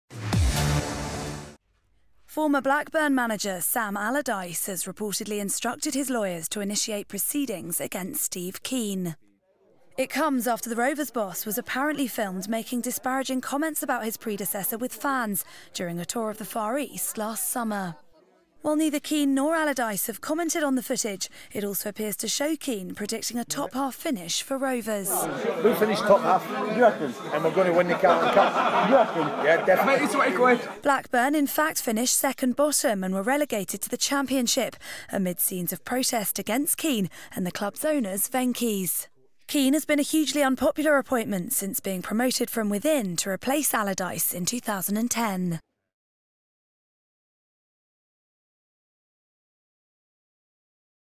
Sam Allardyce 'to sue Steve Kean' over 'disparaging remarks'
Former Blackburn manager Sam Allardyce is reportedly set to sue his successor Steve Kean over apparently disparaging remarks he made last year. Report by Sophie Foster.